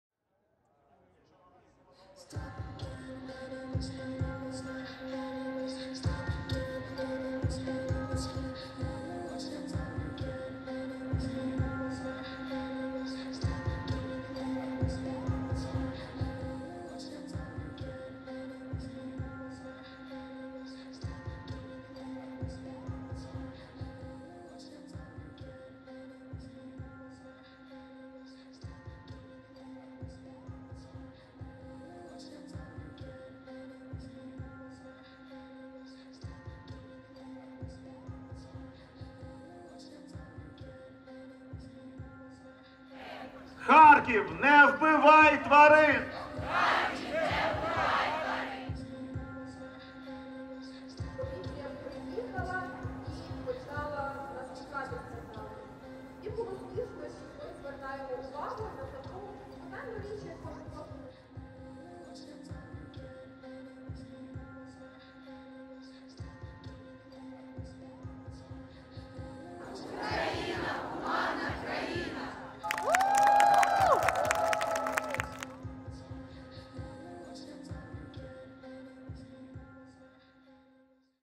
В Україні проходить марш за права тварин
У неділю, 5 вересня, у столиці розпочався Всеукраїнський марш за права тварин під лозунгом «Захист слабких - справа сильних».

Учасники маршу зібралися у парку Шевченка, аби привернути увагу до нагальної потреби захисту тварин.

Вони вимагають покласти край жорстокості та захистити тварин від знущань. На думку активістів, цирки з тваринами, контактні зоопарки, дельфінарії, хутрові ферми та притравочні станції – це варварство, якому немає місця в сучасній Україні.